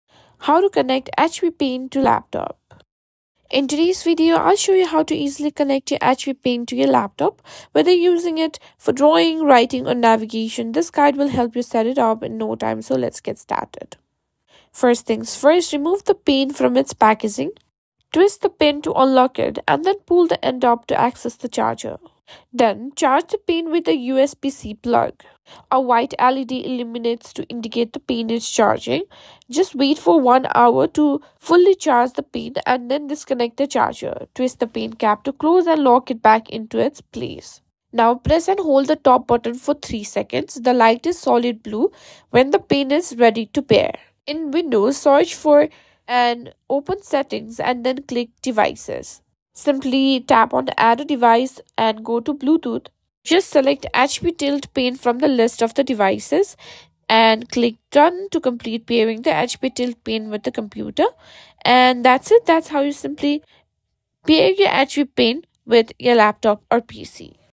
How to Connect HP Pen to laptop
Learn how to easily connect your HP Pen to your laptop in this step-by-step tutorial. Whether you're using an HP touch laptop or a stylus pen on an x360 model, this guide will help you get started with your pen functionality in no time.

In this video, you'll discover how to set up and use your HP Pen on a touch screen laptop. We’ll walk you through the steps to connect your HP Tilt Pen, along with how to start using your stylus pen and operate it on your laptop.

0:20 Step 1: Connecting Your HP Pen
0:45 Step 2: Setting Up Your Pen for Use